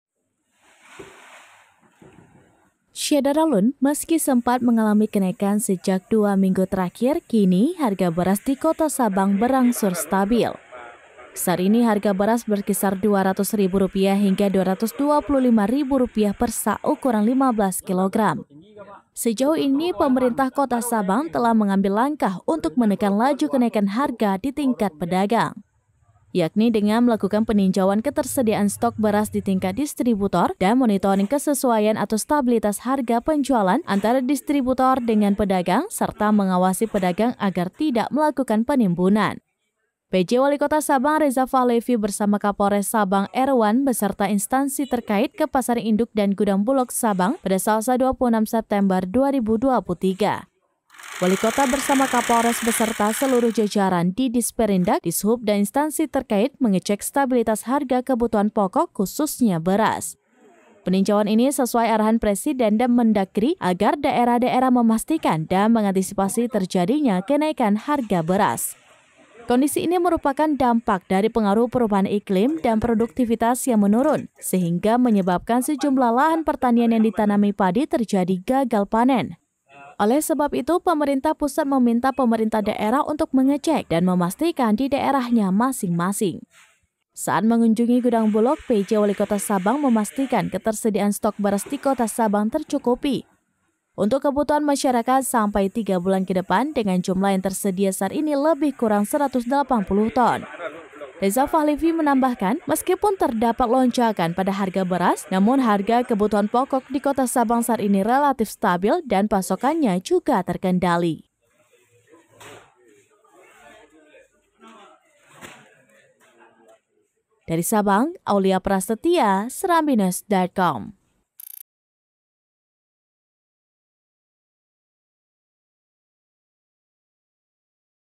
Stok Beras di Kota Sabang Cukup Untuk Tiga Bulan ke Depan
Laporan Aulia Prasetya | Sabang
SERAMBINEWS.COM,SABANG - Meski sempat mengalami kenaikan sejak dua minggu terakhir, kini harga beras di Kota Sabang berangsur stabil. Saat ini harga beras berkisar Rp 200 ribu hingga Rp 225 ribu per sak ukuran 15 kg.

Sejauh ini, Pemerintah Kota Sabang telah mengambil langkah, untuk menekan laju kenaikan harga di tingkat pedagang.

Yakni dengan melakukan peninjauan ketersediaan stok beras ditingkat distributor dan monitoring kesesuaian/stabilitas harga penjualan antara distributor dengan pedagang, serta mengawasi pedagang agar tidak melakukan penimbunan.

Pj Wali Kota Sabang Reza Fahlevi bersama Kapolres Sabang Erwan beserta instansi terkait, ke pasar induk dan Gudang Bulog Sabang, Selasa (26/9/2023).

Wali Kota bersama Kapolres beserta seluruh jajaran dari Disperindag, Dishub, dan instansi terkait, mengecek stabilitas harga kebutuhan pokok, khususnya beras.

Peninjauan ini sesuai arahan  Presiden dan  Mendagri agar daerah-daerah memastikan dan mengantisipasi terjadinya kenaikan harga beras.

Kondisi ini merupakan dampak dari pengaruh perubahan iklim dan produktifitas yang menurun, sehingga menyebabkan sejumlah lahan pertanian yang ditanami padi terjadi gagal panen. Oleh sebab itu, pemerintah pusat meminta perintah daerah untuk mengecek dan memastikan di daerahnya masing-masing.

Saat mengunjungi gudang Bulog, Pj Wali Kota Sabang memastikan ketersediaan stok beras di Kota Sabang tercukupi untuk kebutuhan masyarakat sampai tiga bulan ke depan, dengan jumlah yang tersedia saat ini lebih kurang 180 ton.

Reza Fahlevi menambahkan, meskipun terdapat lonjakan pada harga beras, namun harga kebutuhan pokok di Kota Sabang saat ini relatif stabil dan pasokannya juga terkendali. (*)